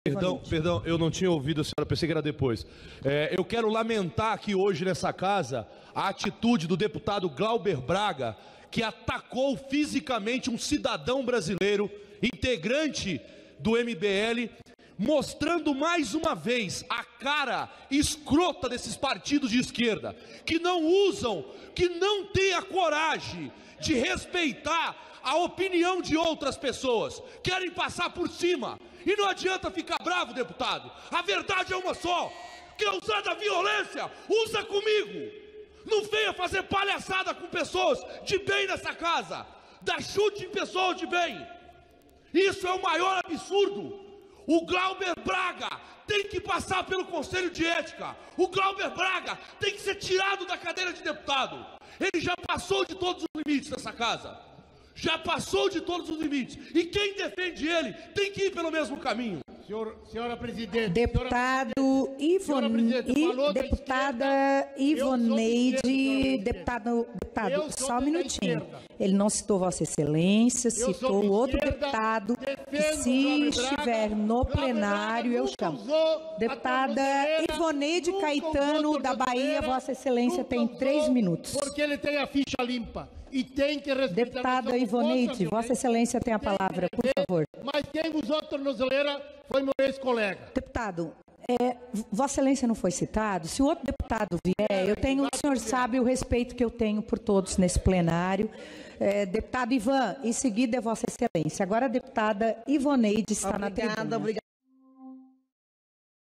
Zé Trovão condena ataque de Glauber Braga a membro do MBL: "Deve ser tirado da cadeira de deputado"
O deputado federal Zé Trovão condenou o ataque de seu colega Glauber Braga a um membro do MBL e afirmou que ele "deve ser tirado da cadeira de deputado".